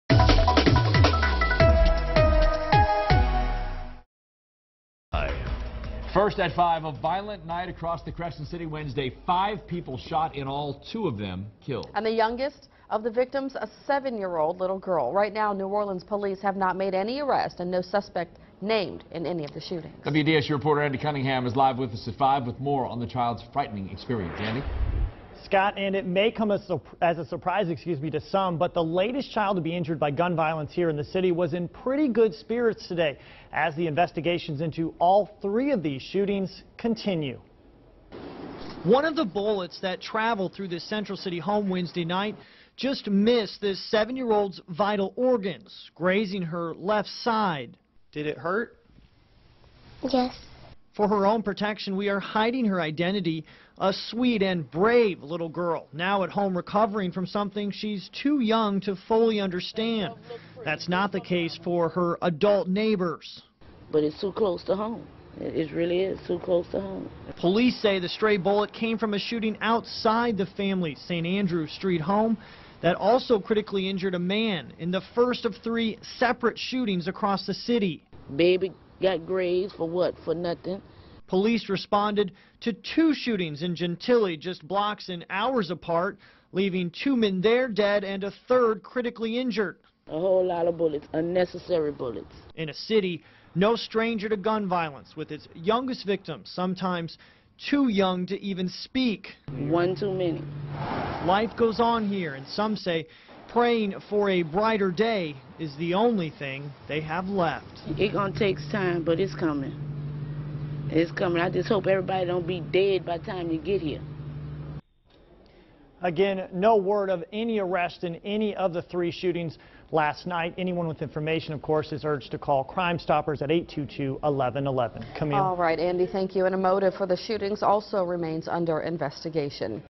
NOPD investigates three shootings that killed 2, injured 3
New Orleans police are investigating three separate shootings that killed two people and left three others, including a 7-year-old girl, injured.